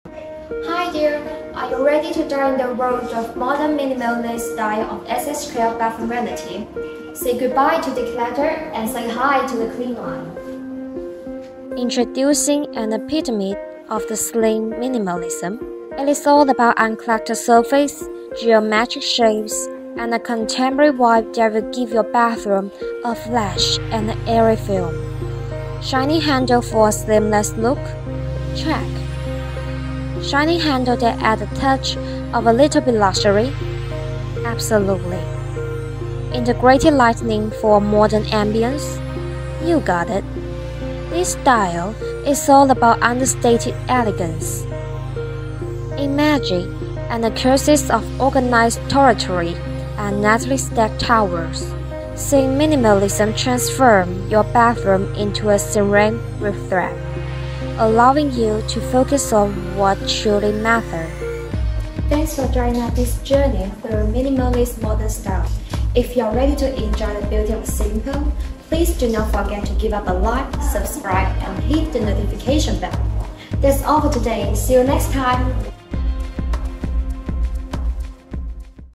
What styles are available for bathroom cabinets? Modern Bathroom Vanity.
Hey there! Today, we're diving into the world of modern elegance style for SHKL bathroom vanity.